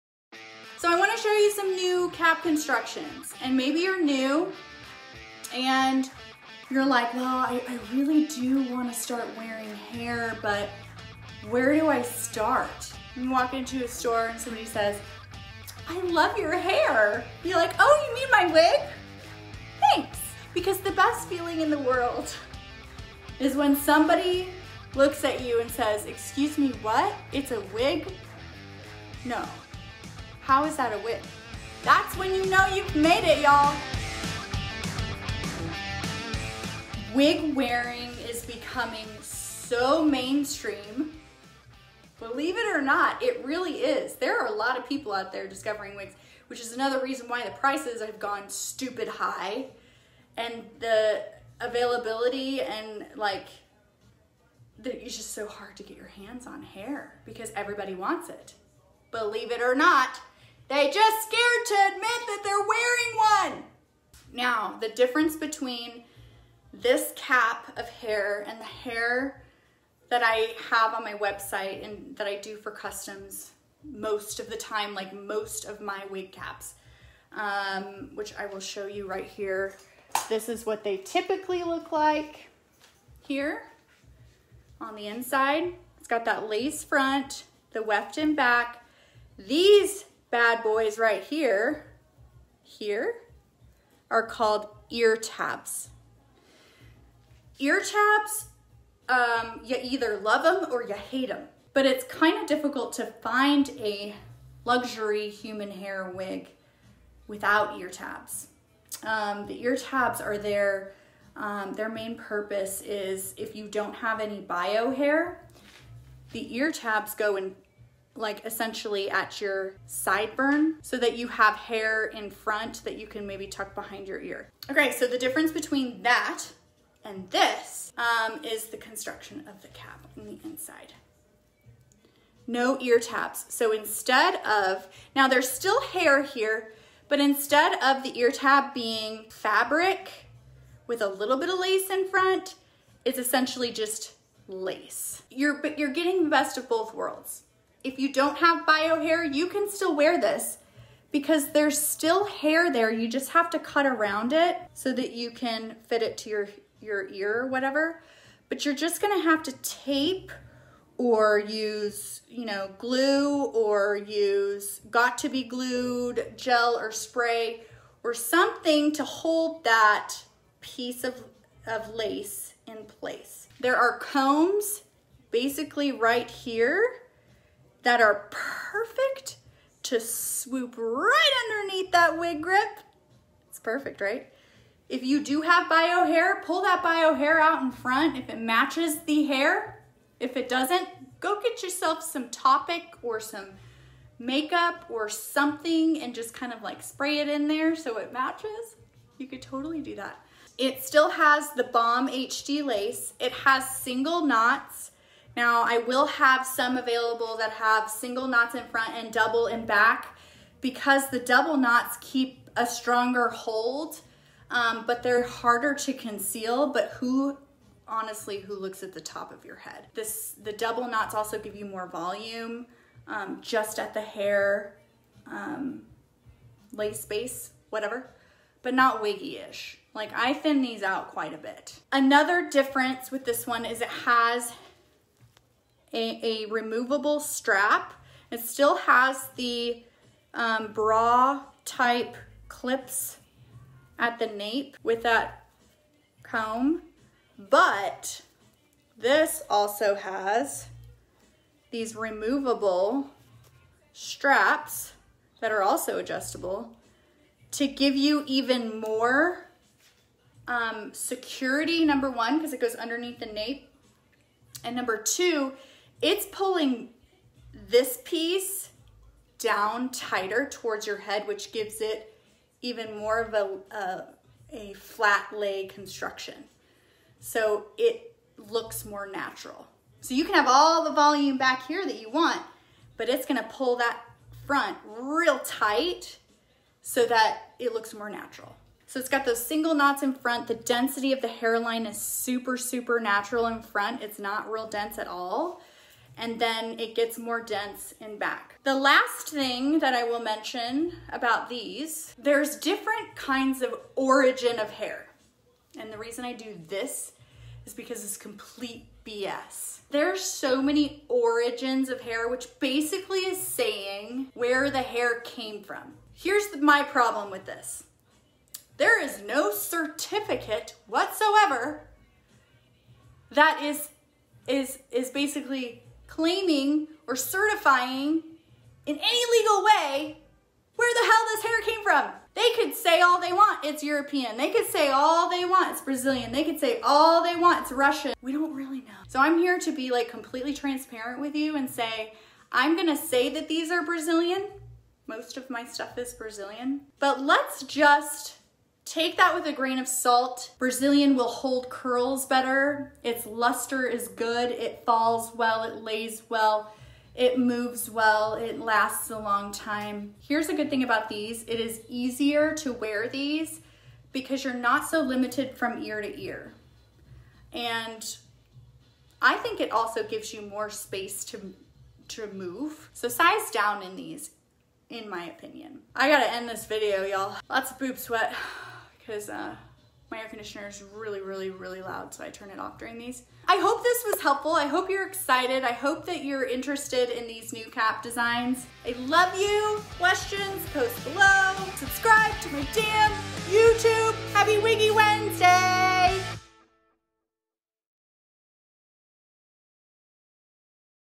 SECRET! I Wear Wigs | Where to Find the Best Cap Construction
So realistic no one will know it's a wig!

I will tell you all about the two styles of wig caps used in my line of luxury human hair wigs I will explain the pros and cons of each to help you decide which is the best option for you.

These wigs are so realistic and so comfortable that you will forget you're wearing a wig and strangers won't believe it's not real.

Plus, behind the scenes daily shenanigans in the she shed.